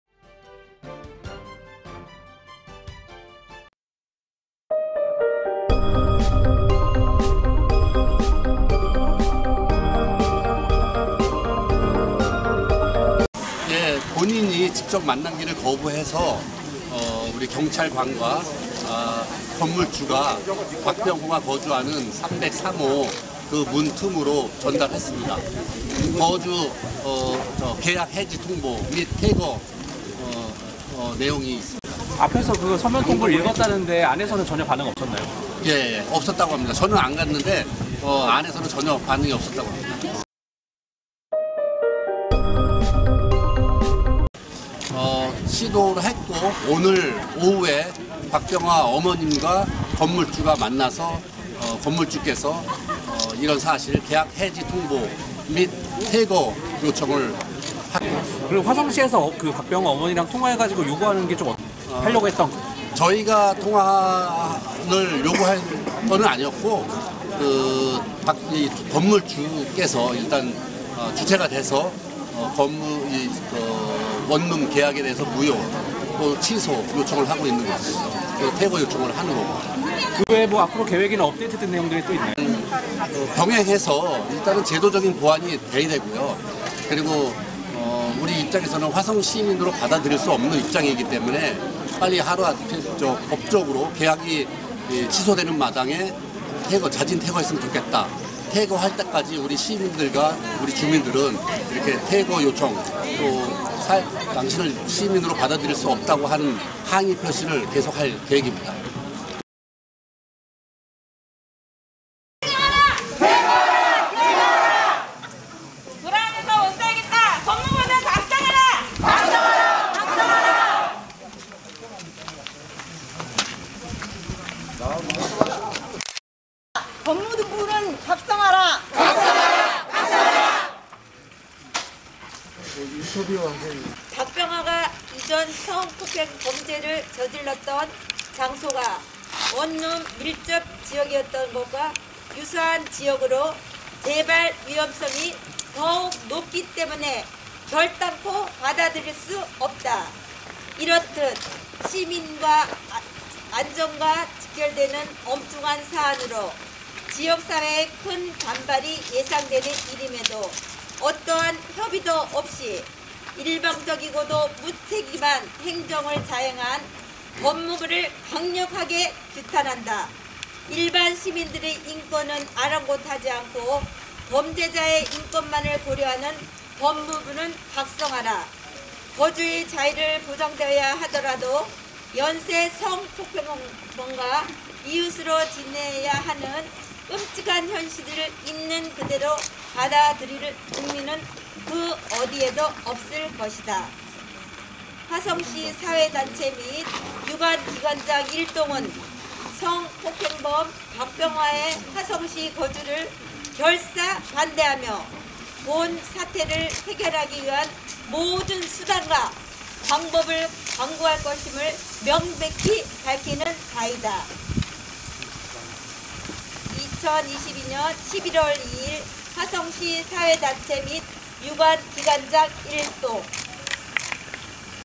성범죄자 박병화는 화성시를 떠나라!
[양해용 기자]=성범죄자 박병화는  화성시에 거주할 자격 없다고 화성시민은 규탄대회로 연일 참여하고 있다.